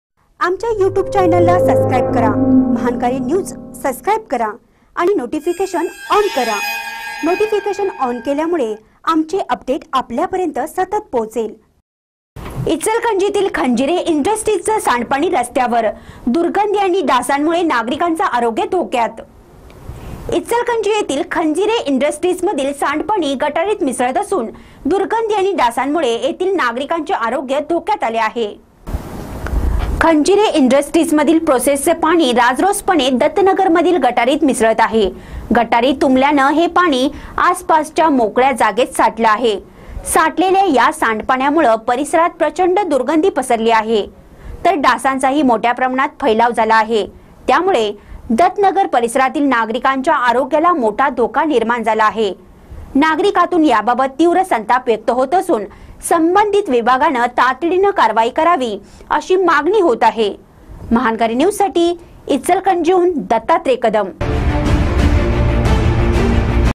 इचलकरंजीतील खंजिरे इंडस्ट्रीजचे सांडपाणी रस्त्यावर
इचलकरंजीतील खंजिरे इंडस्ट्रीजचे सांडपाणी रस्त्यावर : दुर्गंधी आणि डासांमुळे नागरिकांचं आरोग्य धोक्यात